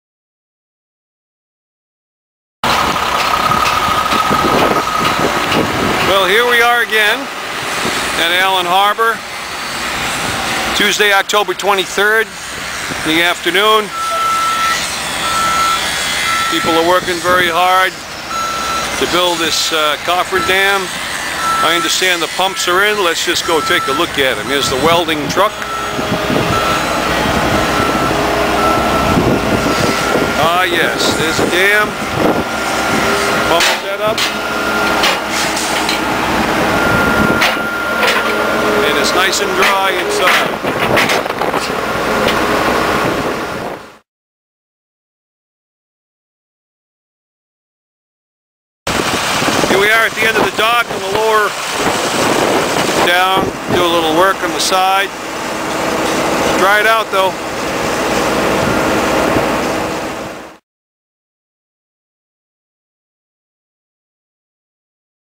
Boat Ramp Repair - Allen Harbor
Robert Our Co. performing contract at Allen Harbor in Harwich Port, Massachusetts where they have built a coffer dam to dry out and replace the bottom section of boat ramp. This is an area that was underwashed by lazy boaters that feel it is there privilege to "powerload" there boats onto the trailers. This activity undermines boat ramps everywhere and needs to stop!